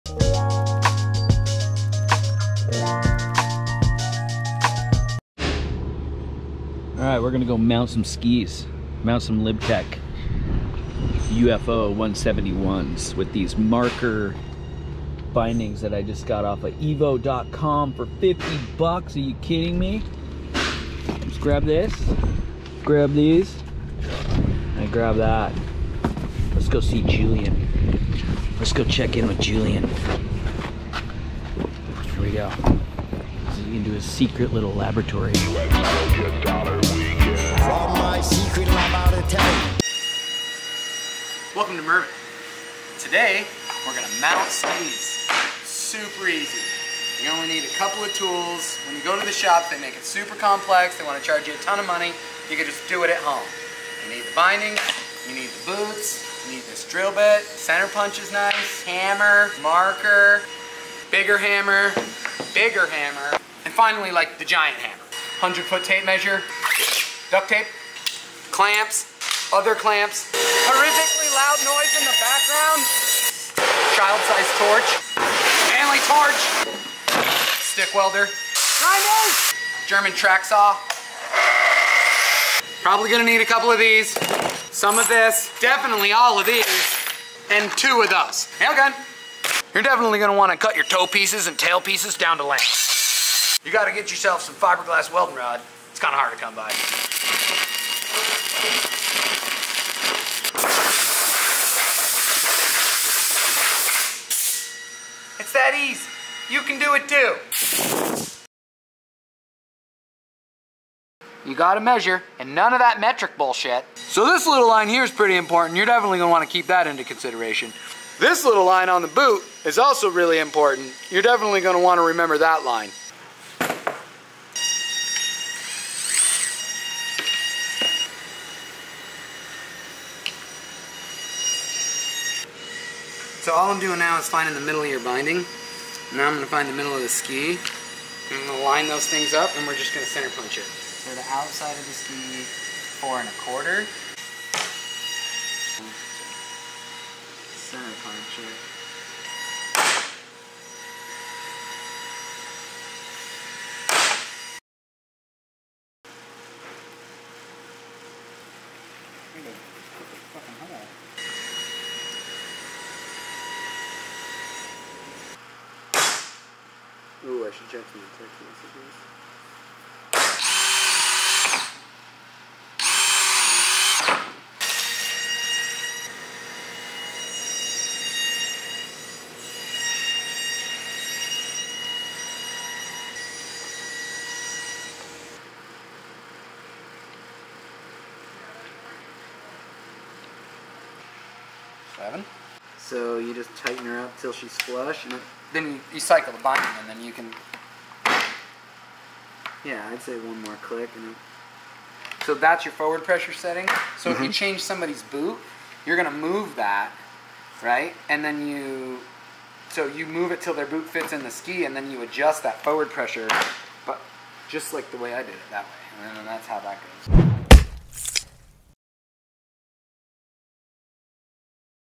MOUNTING YOUR OWN SKIS?????
Here's a funny little take on what you'll need while mounting your own skis. Pro tips from Mervin MFG.'s very own Julian! Mount at your own risk.